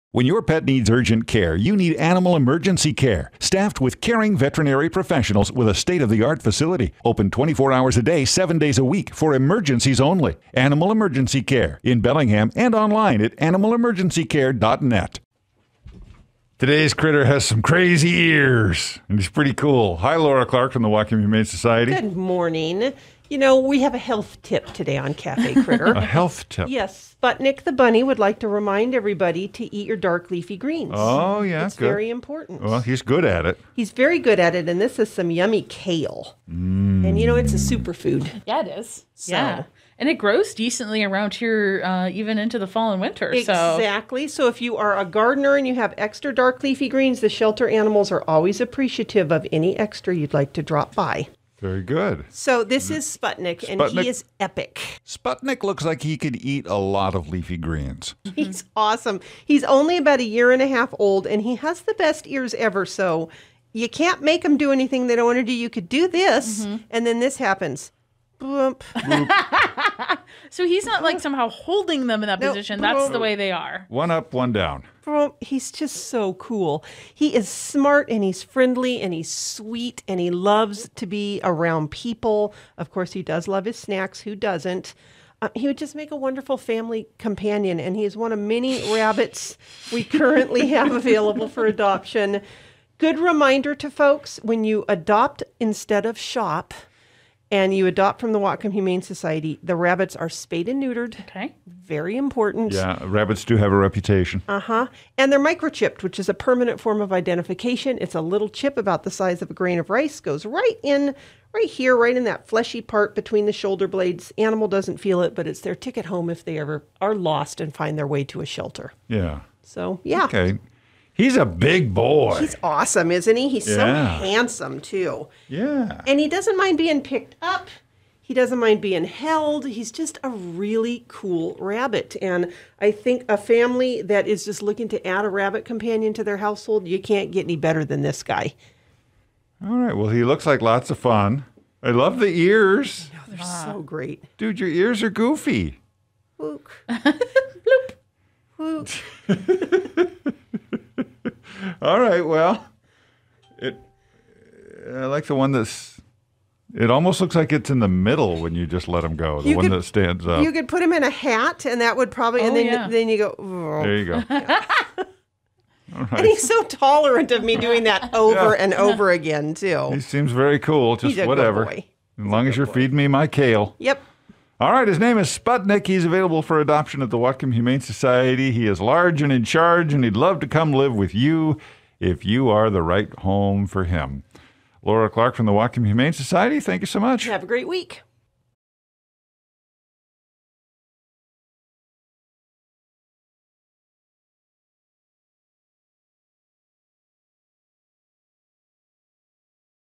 KAFE Kritter Sputnik 09 29 21
Our KAFE Kritter of the Week: Sputnik, the bunny with the crazy ears